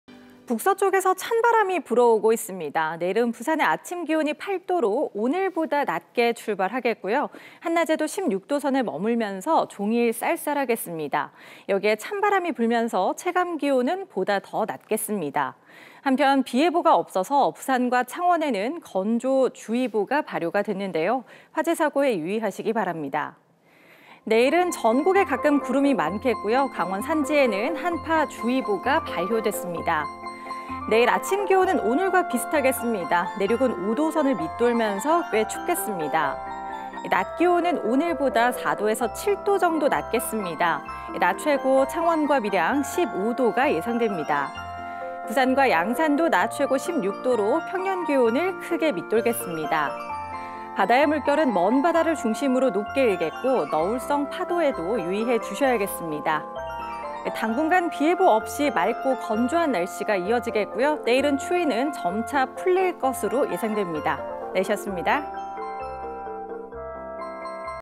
북서쪽 찬바람 불어..기온 뚝
북서쪽에서 찬바람이 불어오고 있습니다.
내일은 부산의 아침 기온 8도로 오늘보다 낮게 출발하겠고요,
한낮에도 16도선에 머물면서 종일 쌀쌀하겠습니다.
여기에 찬바람이 불면서 체감기온은 보다 더 낮겠습니다.
한편 비예보가 없어서 부산과 창원에는 건조주의보가 발효됐는데요,
화재사고에 유의하시기 바랍니다.

내일은 전국에 가끔 구름이 많겠고요,
강원산지에는 한파주의보가 발효됐습니다.

내일 아침 기온은 오늘과 비슷하겠는데요,
내륙은 5도선을 밑돌면서 꽤 춥겠습니다.

낮 기온은 오늘보다 4도에서 7도 정도 낮겠습니다.
낮 최고 창원과 밀양 15도가 예상됩니다.

부산과 양산도 낮 최고 16도로 평년기온을 크게 밑돌겠습니다.

바다의 물결은 먼바다를 중심으로 높게 일겠고,
너울성 파도에도 유의해주셔야겠습니다.

당분간 비예보 없이 맑고 건조한 날씨가 이어지겠고요,
때이른 추위는 점차 풀리겠습니다.